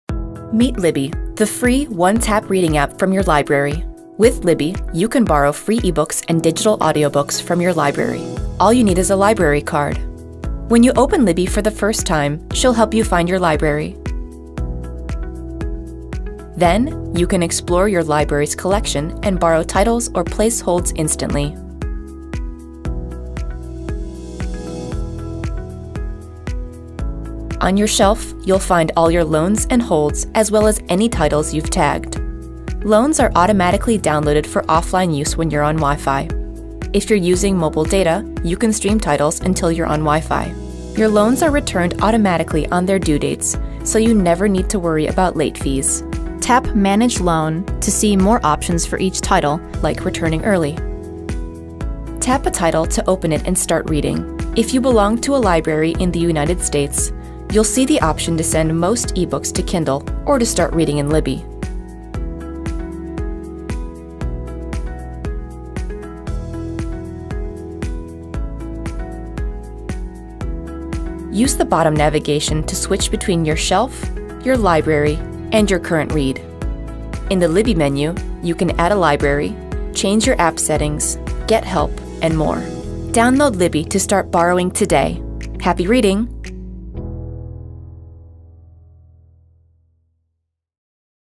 Getting Started with the Libby App
This video provides a brief overview of the Libby app, powered by OverDrive.  For more information about DPL's digital resources,